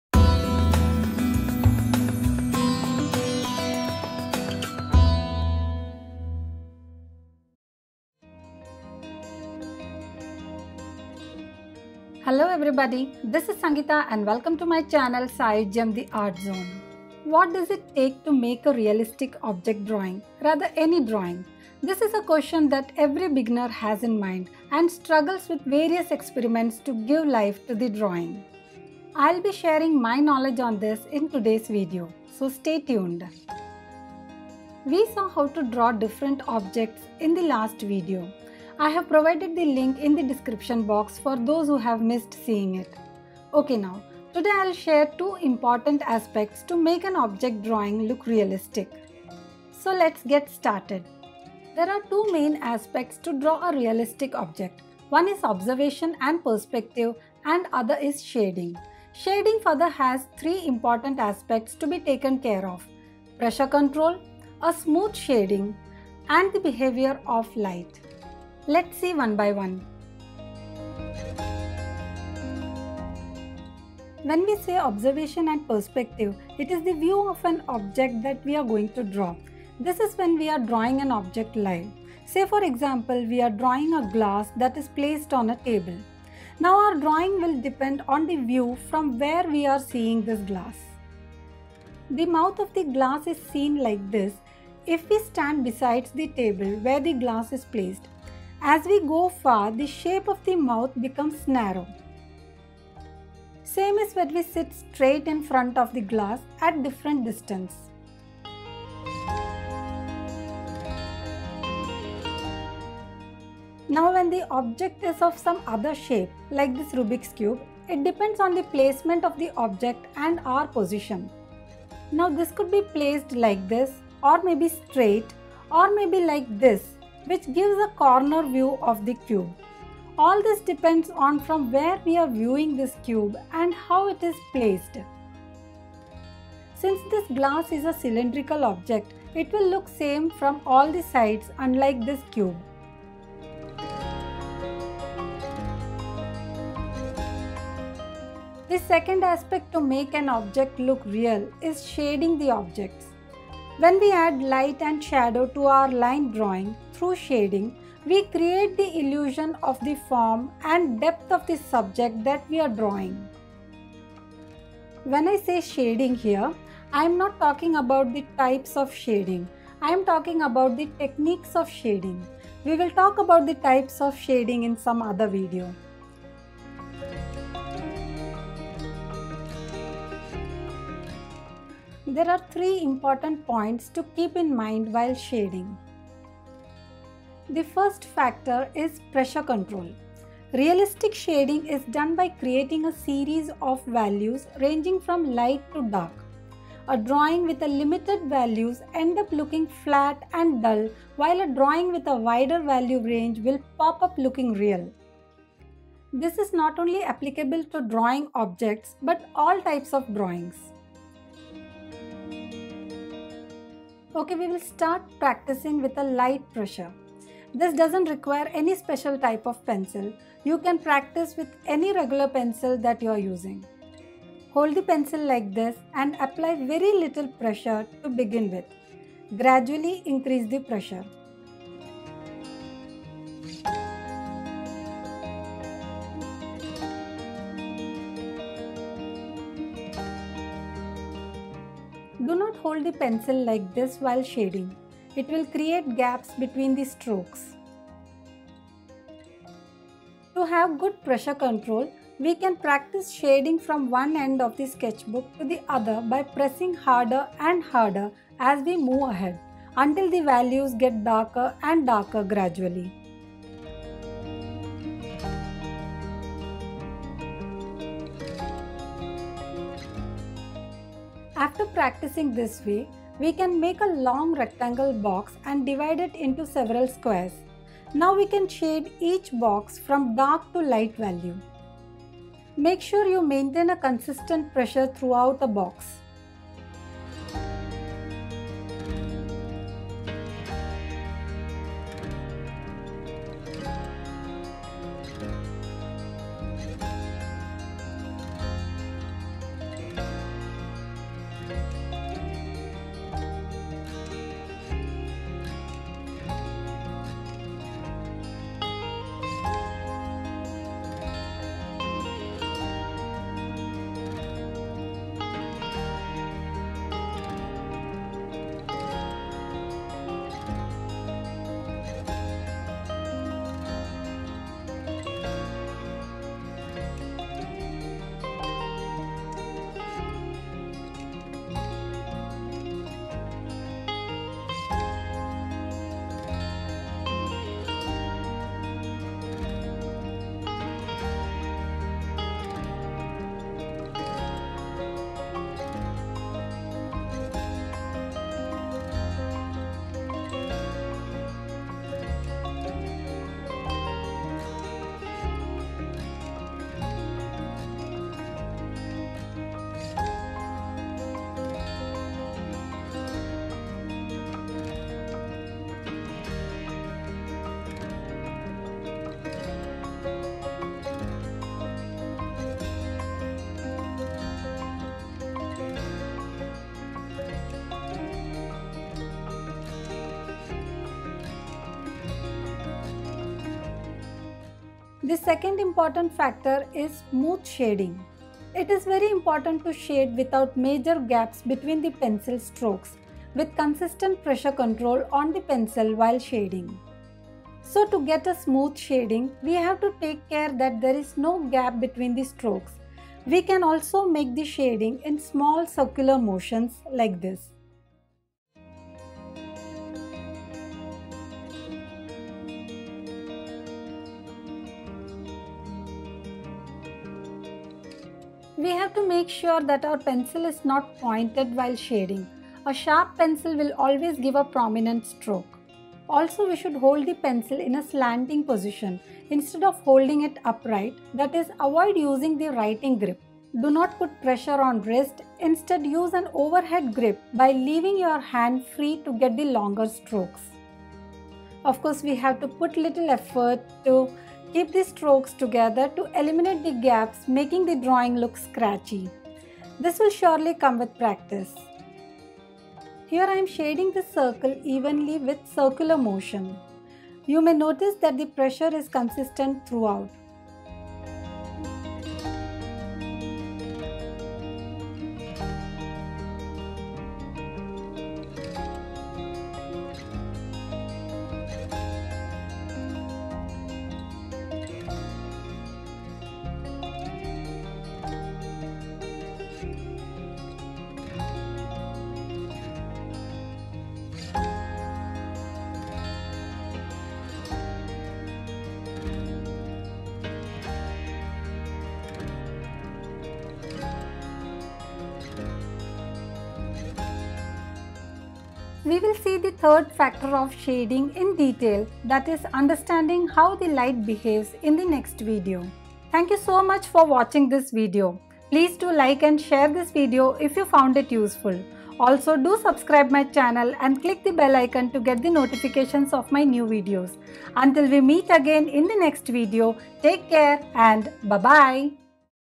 Object Drawing and Still Life Drawing - 3 | How To Draw A Realistic Object | Saayujyam-The Art Zone
A complete series of drawing tutorials for all the beginners. Now you can learn how to draw by sitting at home.
This video is on drawing the basic structure of some objects in Object Drawing and Still Life Drawing.

saayujyam-theartzone